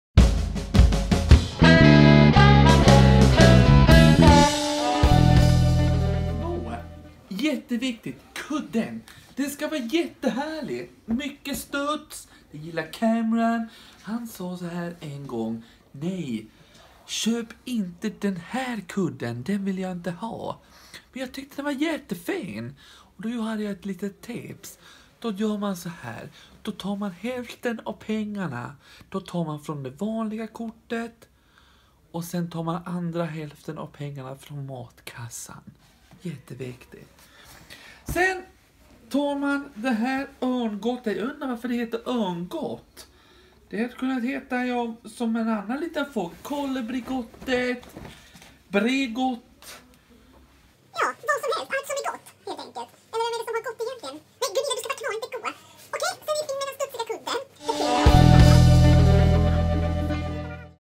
Marias och Gunillas Roadtrip - Maria bäddskola del 2 PARODI
Avsnitt 16 av Hollywoodfruarna Maria Montazamis och Gunilla Perssons roadtrip genom Sverige.  Maria fortsätter sin bäddskola och kommer med ett väldigt viktigt husmorstips.

Magnus Carlsson är komiker, skådespelare, artist, programledare, producent och podcastare.